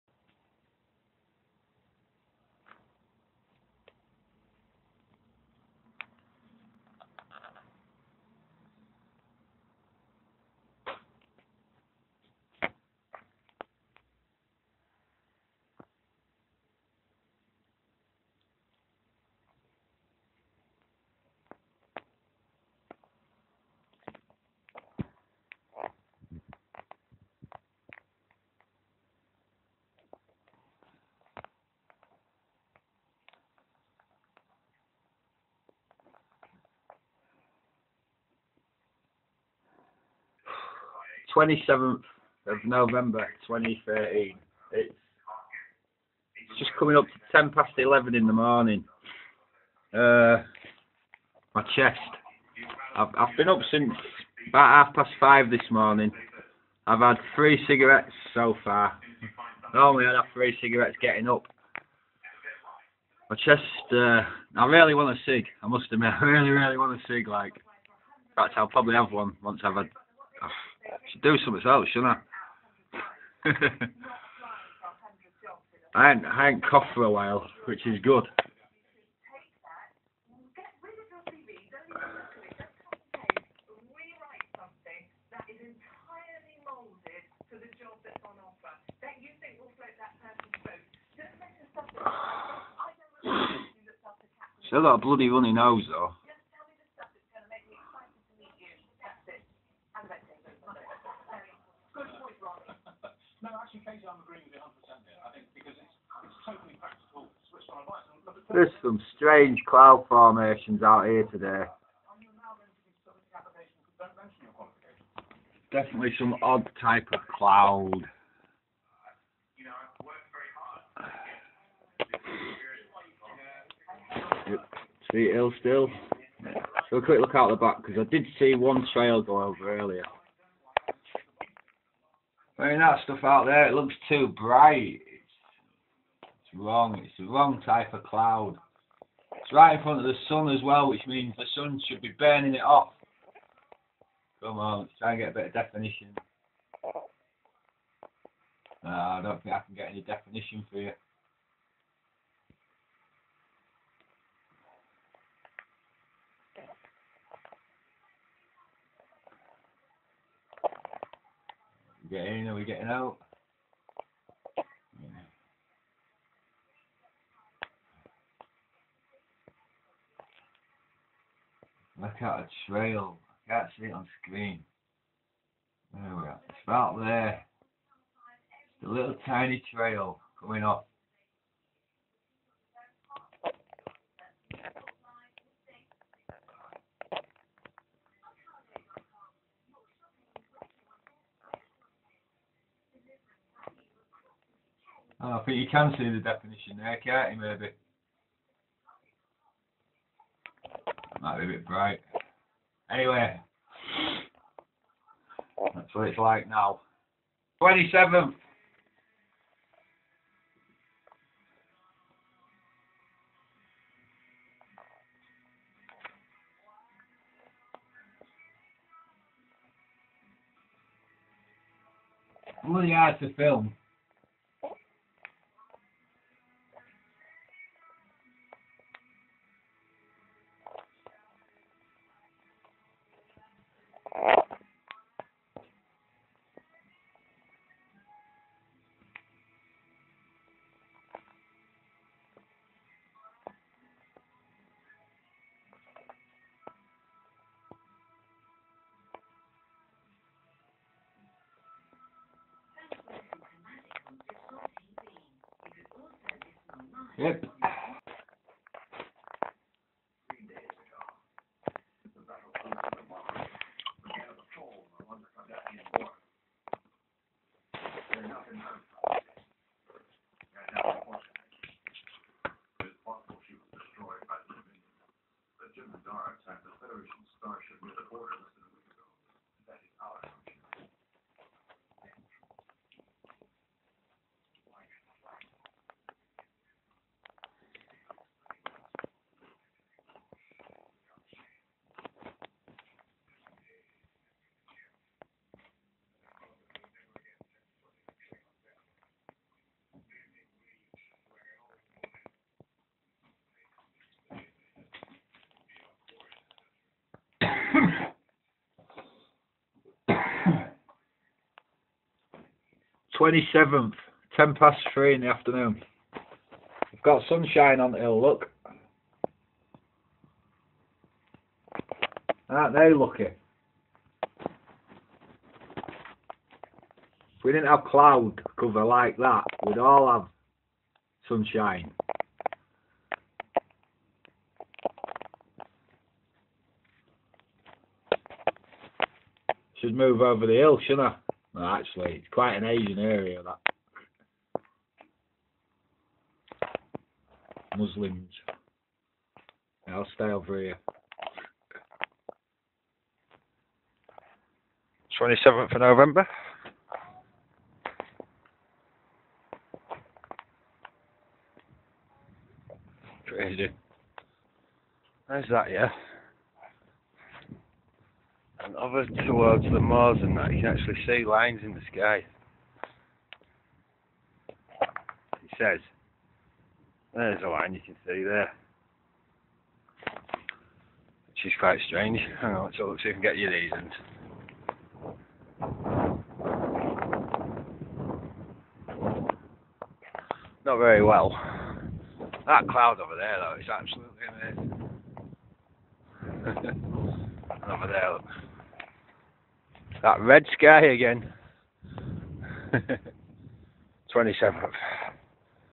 Cloudy Weather 27th Novemeber 2013
Crazy looking cloud cover.Warm throughout day.